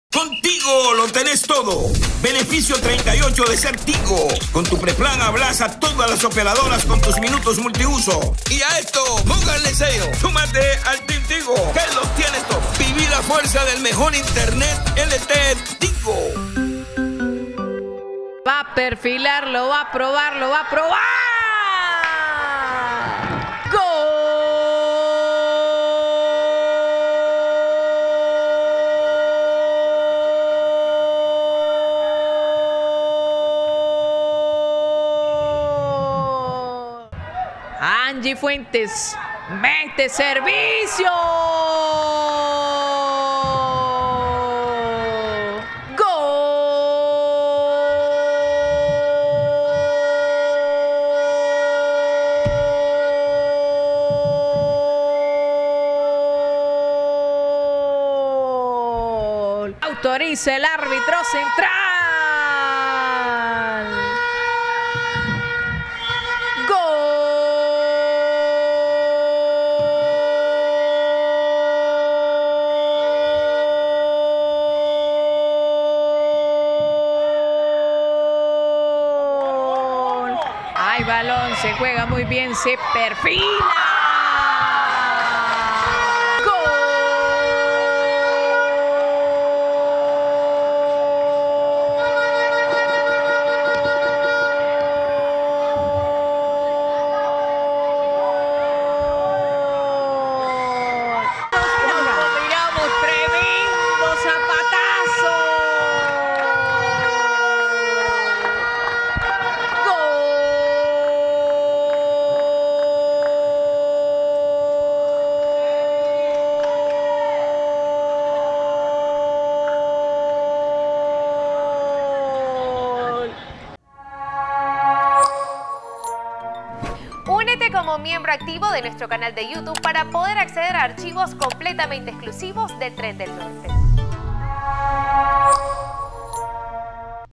🎥 RESUMEN | Real Estelí U15 Femenil 5️⃣🆚0️⃣ Madriz U15 Femenil | Apertura 2022 Categorías Menores
Torneo Apertura 2022 - Campeonato Nacional de Categorías Menores - Futbol de Nicaragua.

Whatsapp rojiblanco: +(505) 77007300